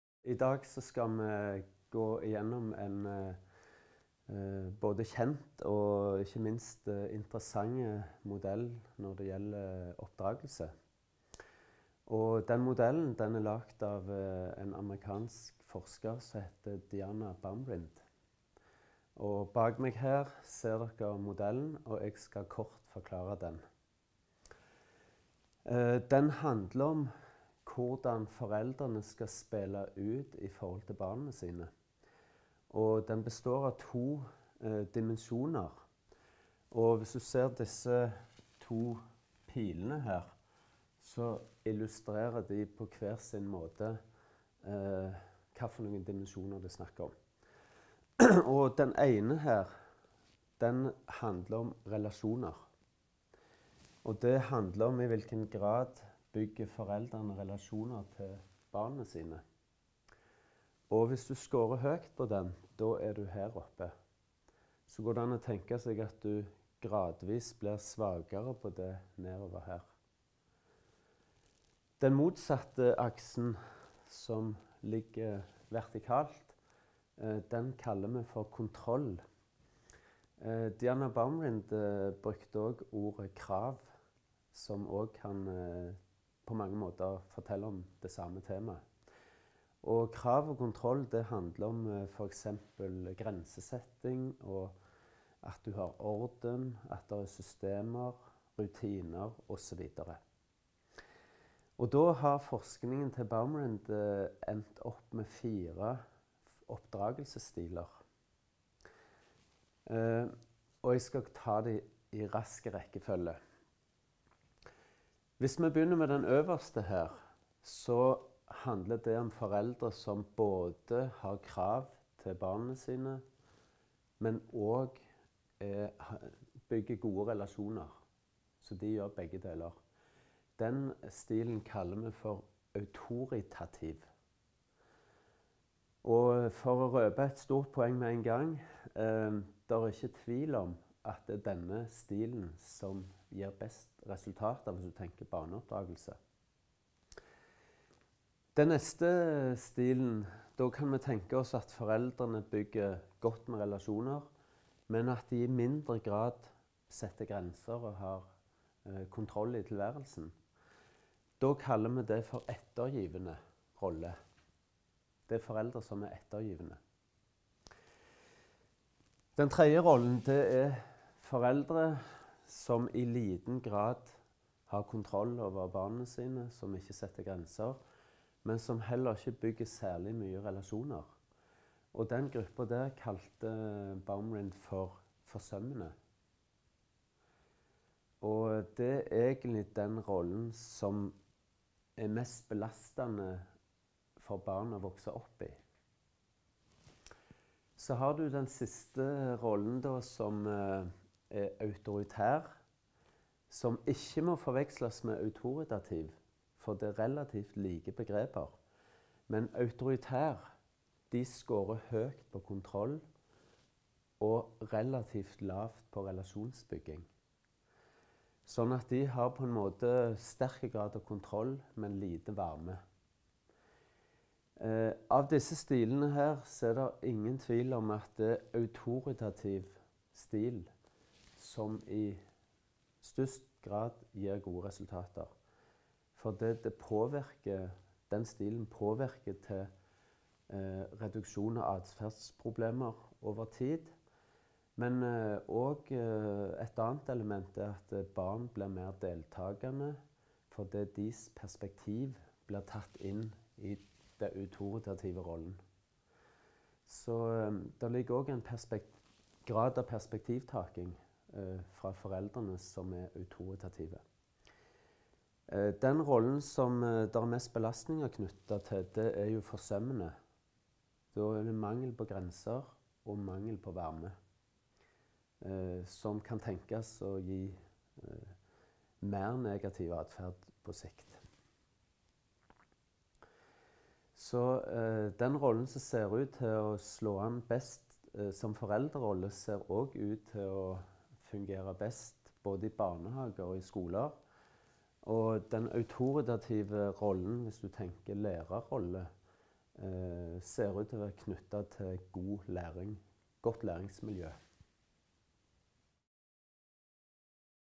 Ulike oppdragelsestiler
Førsteamanuensis Pål Roland ved Læringsmiljøsenteret (UiS) forklarer modellen som omhandler autoritative voksne. Det er en kjent teori om oppdragelsesstiler som er utviklet av Diana Baumrind (1991)